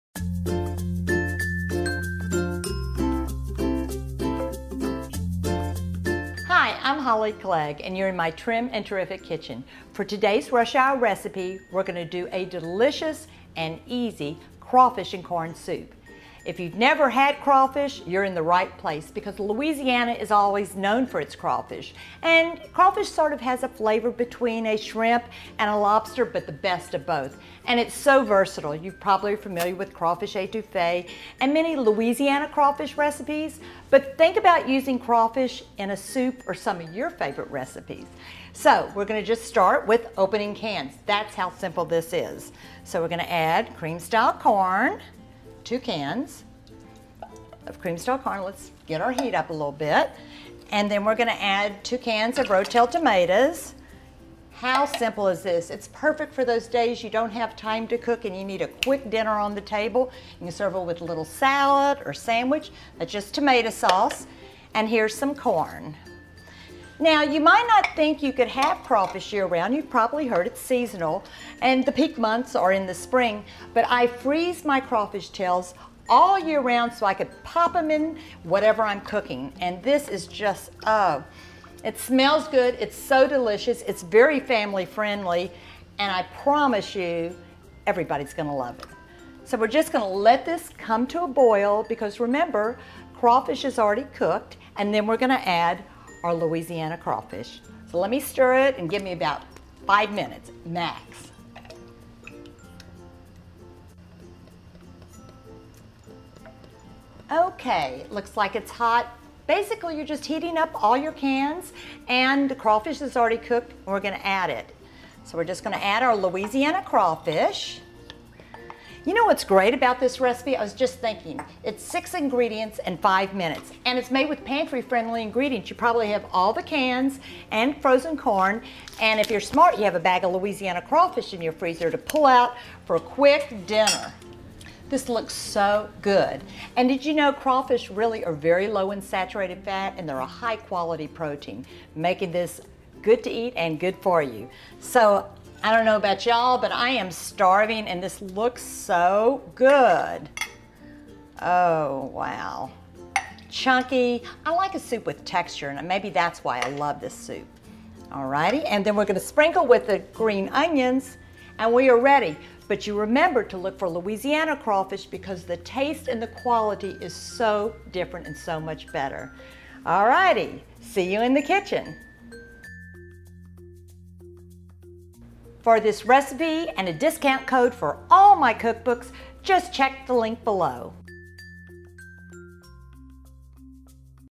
Louisiana Crawfish Corn Soup- 5 Minute Easy Cajun Crawfish Recipe
Try this amazing and easy Lousiana recipe for Crawfish Corn Soup made with Louisiana Crawfish, best flavors of both shrimp and lobster - in this quick and so simple easy crawfish bisque style healthy recipe - one-pot meal Crawfish Corn Soup.  Only 6 ingredients so if you can open cans, and add a few fresh ingredients, you have made the fastest family friendly homemade meal!  And, if you have leftover boiled crawfish, you have the bets seasoned crawfish for the soup ever!